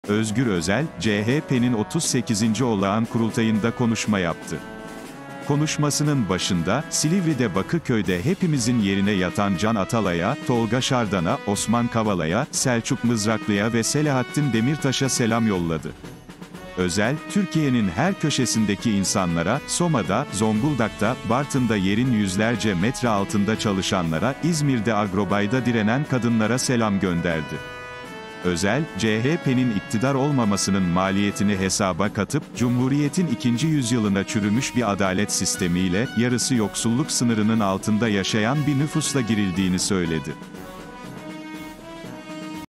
ÖZGÜR ÖZEL, KURULTAY'DAKİ KONUŞMASINA CEZAEVİNDEKİ 4 İSME SELAM YOLLAYARAK ...
Özgür Özel, Kurultay'daki konuşmasına cezaevindeki 4 isme selam yollayarak başladı